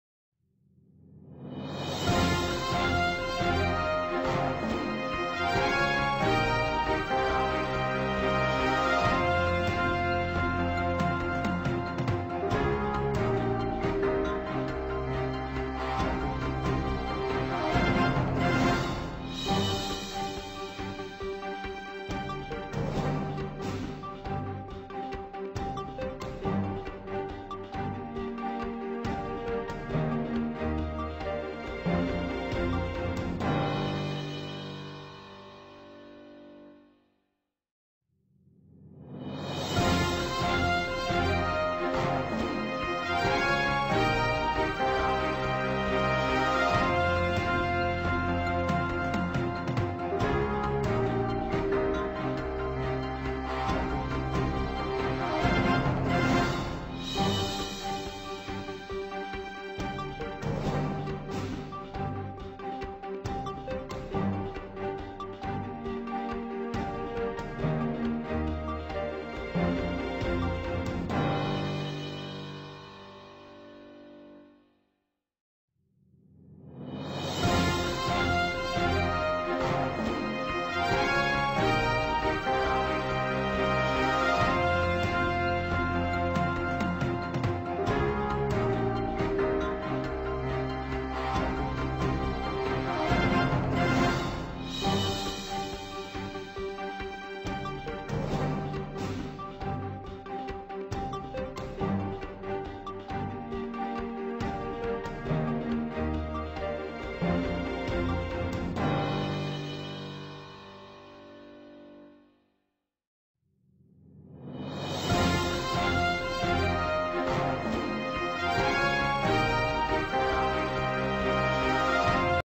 તલાટી/કલાર્ક પરીક્ષા ક્યારે યોજાશે?કેટલા ફોર્મ ભરાયા?આજનો તા.28/2/2019 નો ન્યુઝ રીપોર્ટ
*પંચાયત ના તલાટી-ક્લાર્ક ની પરીક્ષા તારીખ અંગે આજનો ન્યુઝ રીપોર્ટ*